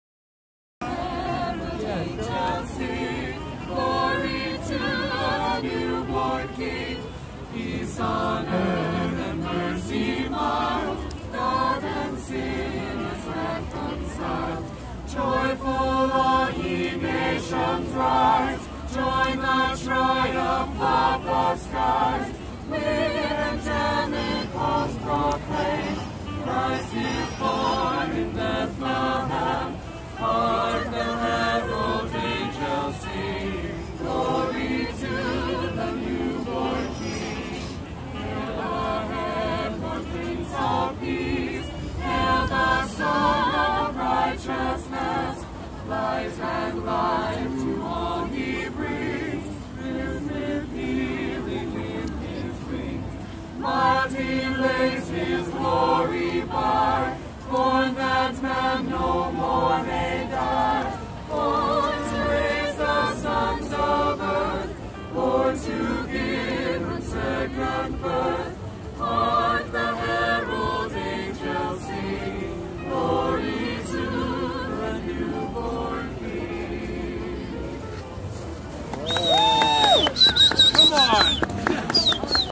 Callbacks sing Christmas Carols
at Bryant Park in NYC on Dec.14th 2013